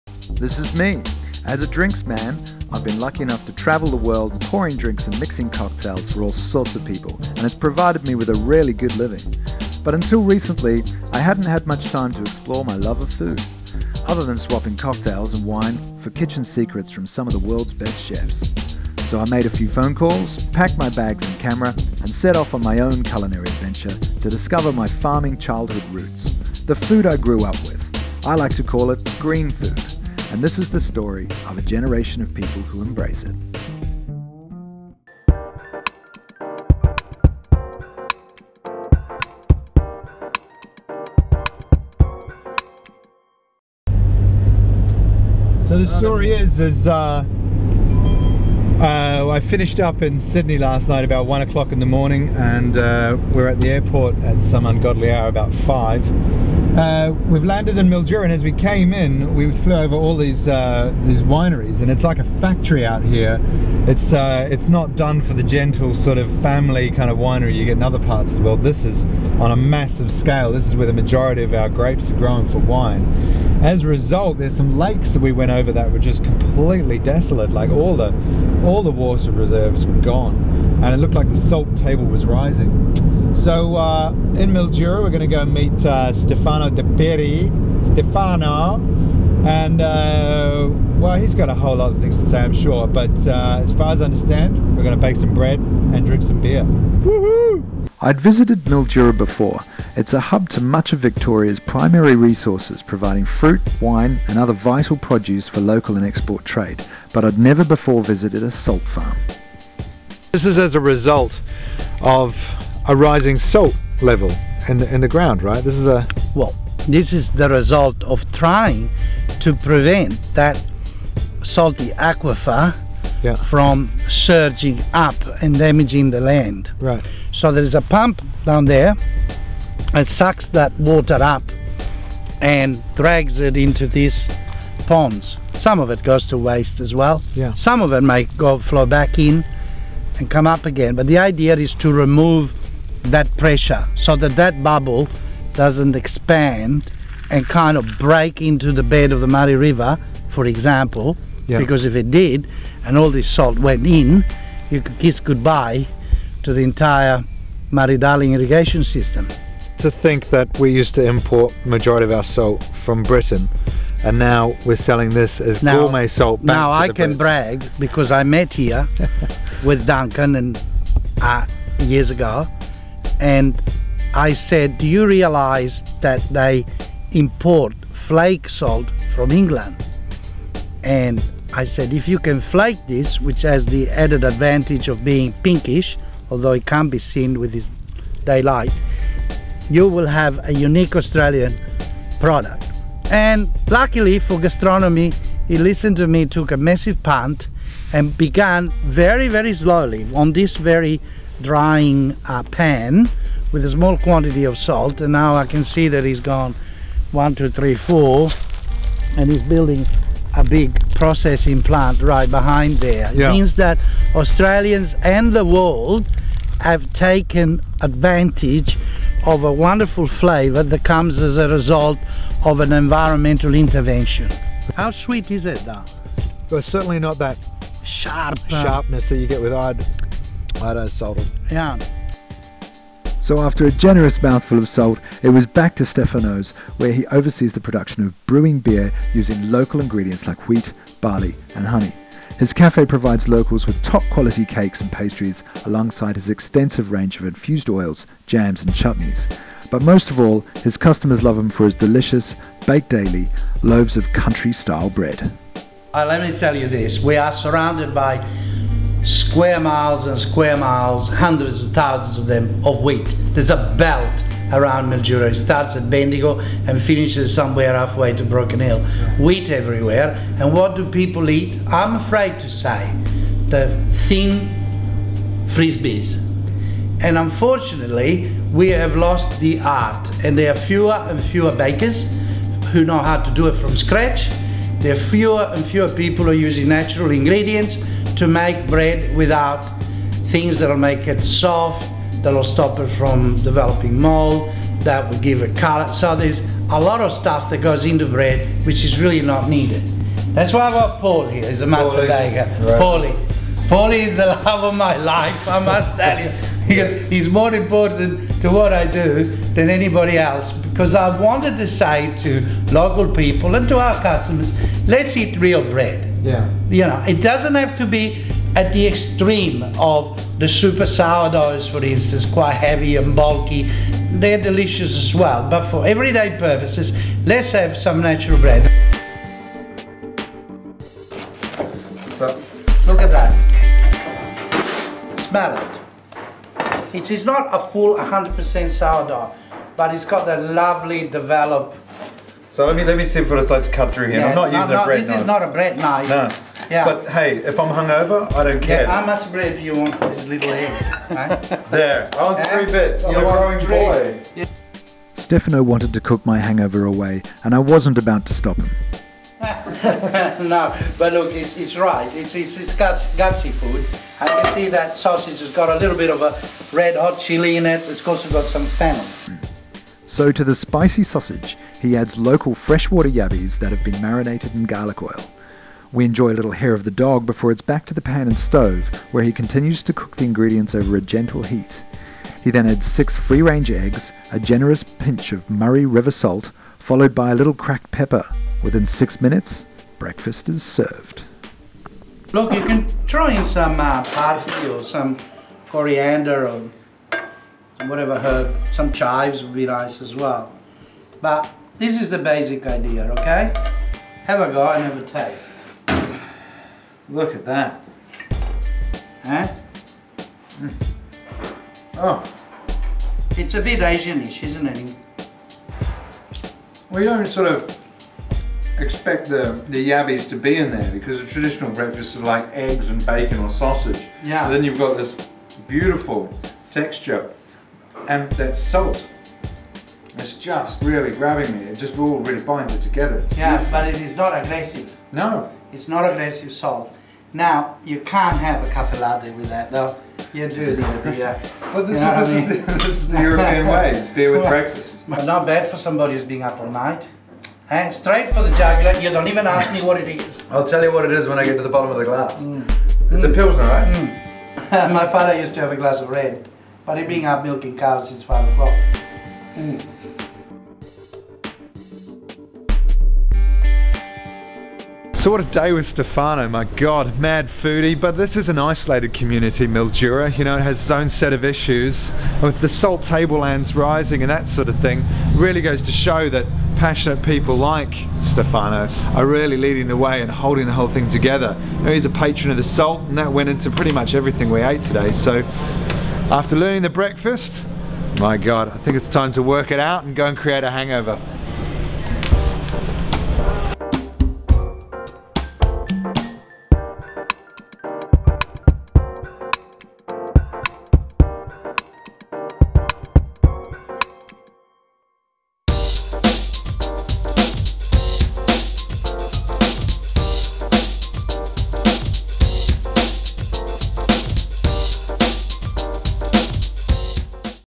Green Food Generation_Stefano's
Woody's culinary adventure took him to Mildura to meet with Stefano di Pieri. It was a day filled with food, beer and wine but above all the experience to hang out with one of Australia's most celebrated and passionate chefs was something unforgettable.